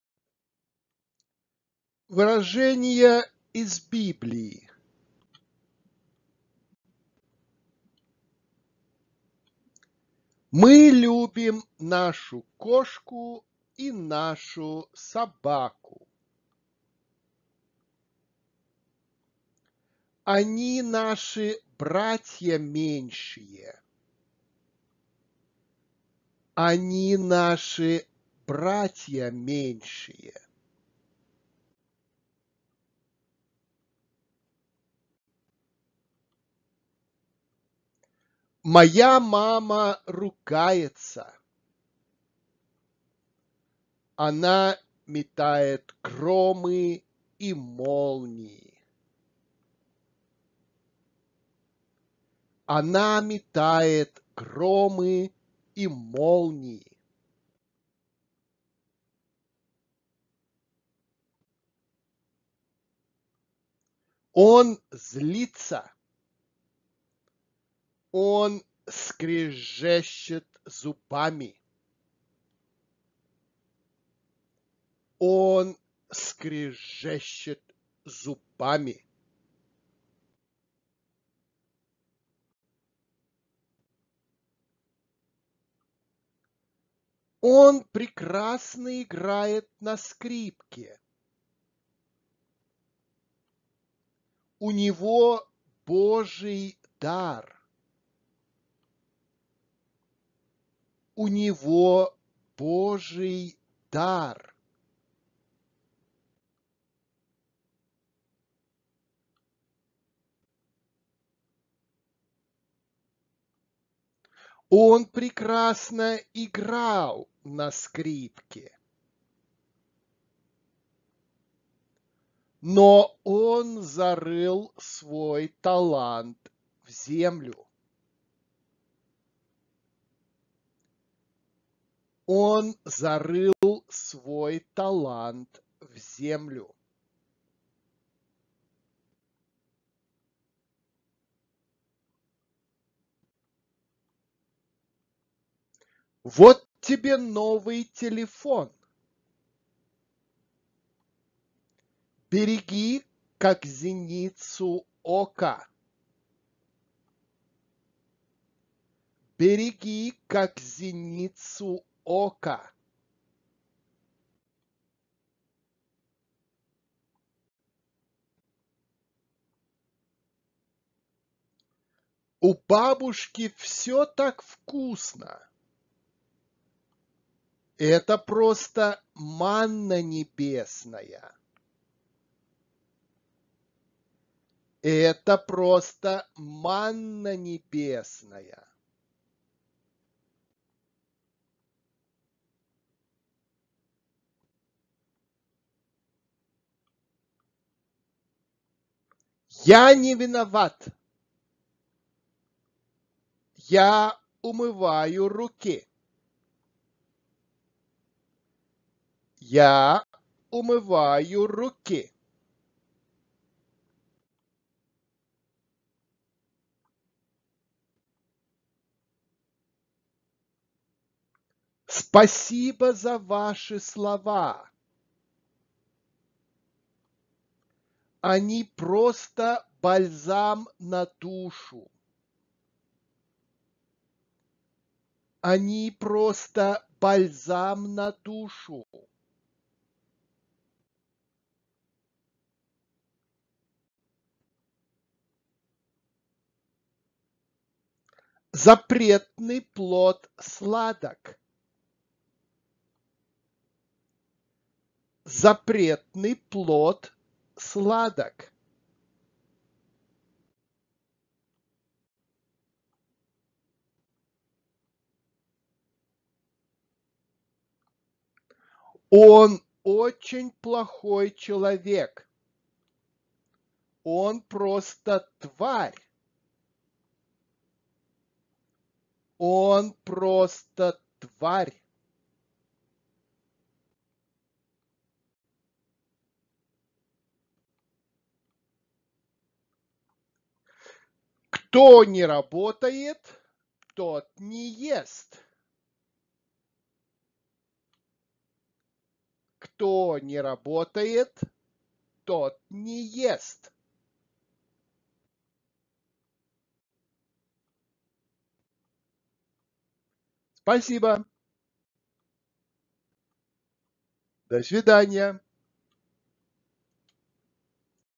Russian language: Biblical phrases
In this series, I will teach you the core Russian words that you must know if you're a an absolute beginner.
I recommend, after you’ve learned the new words, from time to time review what you learned in previous episodes. Reviewing is one of the most important parts of learning a language!
All vocabularies are divided into topics, so you may skip an episode if your are not interested in this particular topic. Please say all words loudly so that your can improve both your reading skills and your pronunciation.
Please write in comments what vocabularies else you would like to learn?